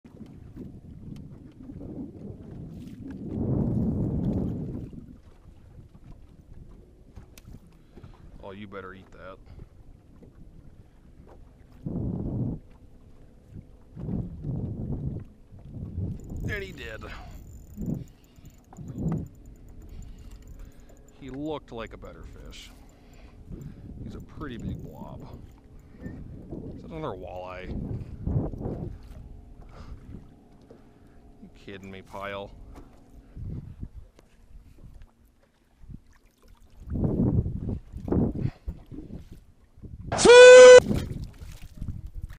Livescoping walleyes 100% on accident :)
Looking for smallmouth and what do you know... Walleyes